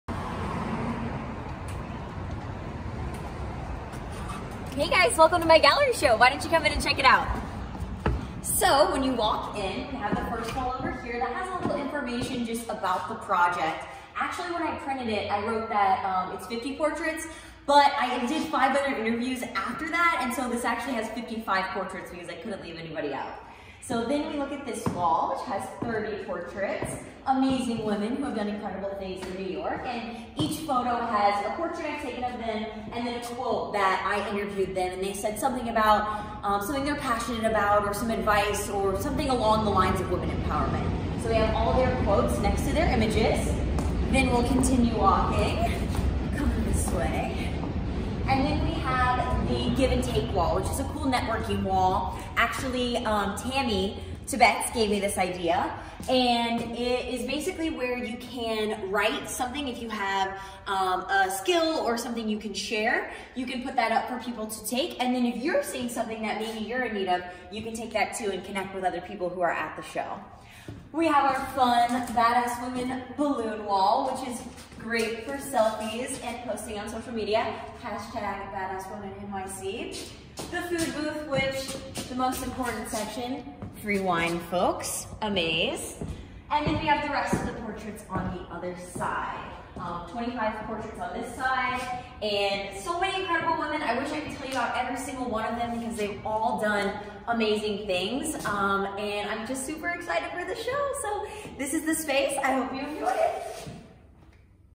A Tour of the Badass Women Art Gallery Show in NYC!
In January 2019, Gaby started a portrait project documenting the stories and photographing incredible women entrepreneurs and business owners in New York City who are doing great things and making a difference. This is the resulting gallery show, held in Brooklyn NYC on March 7, 2020 (just in time for International Women's Day!). Have a look at the setup.

You can find more information about the project or by visiting Gaby on Instagram @gdeimz. Huge thank you to everyone who came to the show and supported the project!